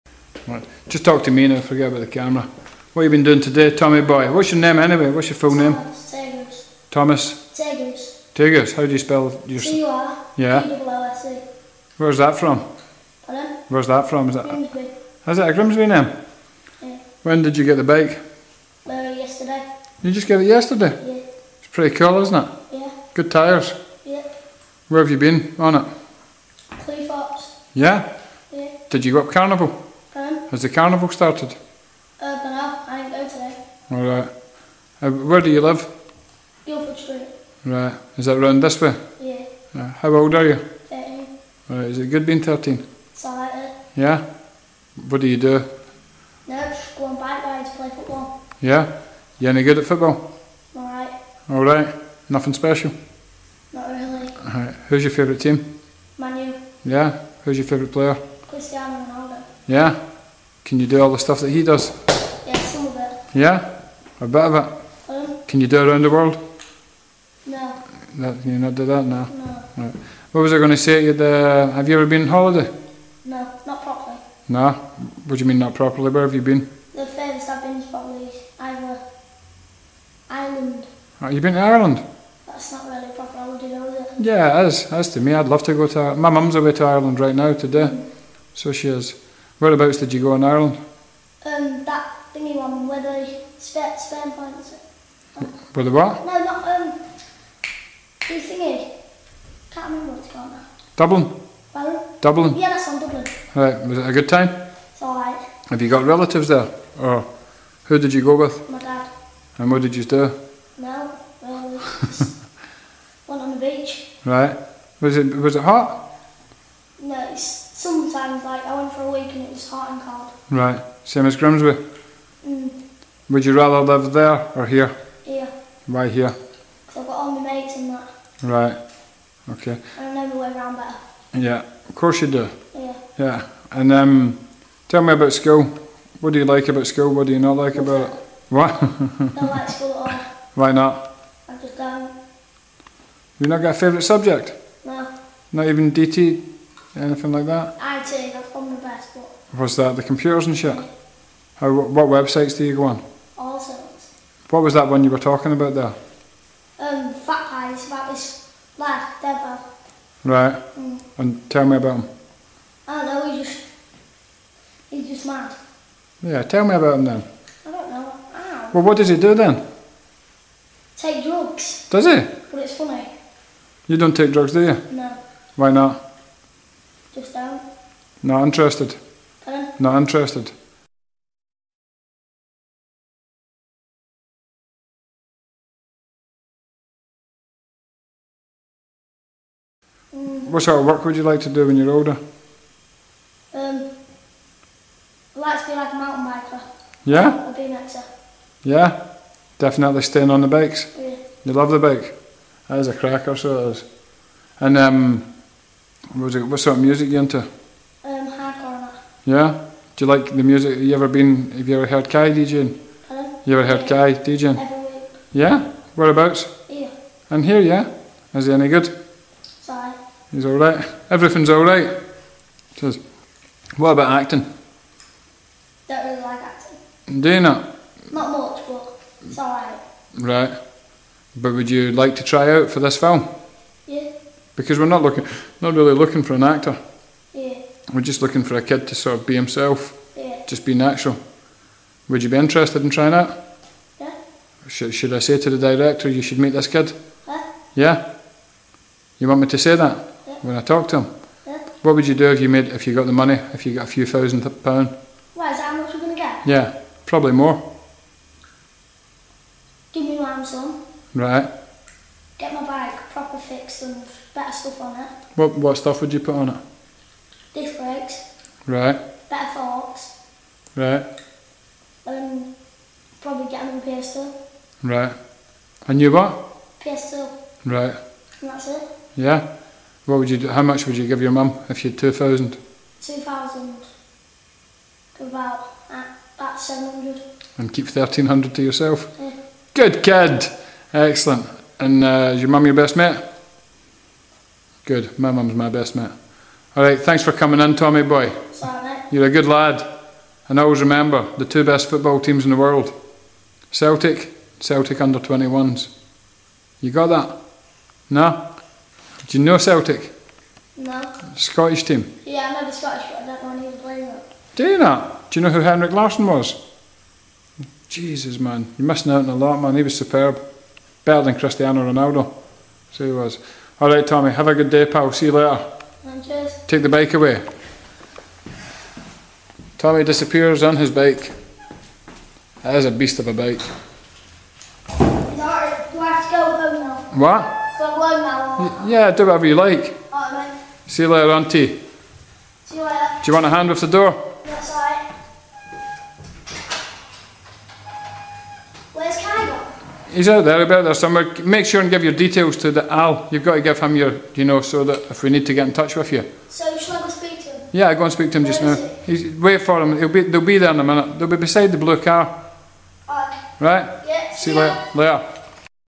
Thomas Turgoose first audition
Tape used for This is England casting.